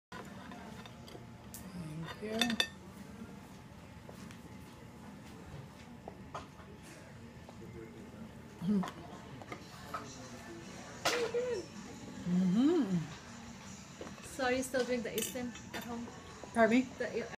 Eileen Reedy Eating Independently After Spinal Cord Injury
Eileen Reedy using a fork to eat independently after a spinal cord injury. Eileen fundraises for uninsured expenses with the nonprofit HelpHOPELive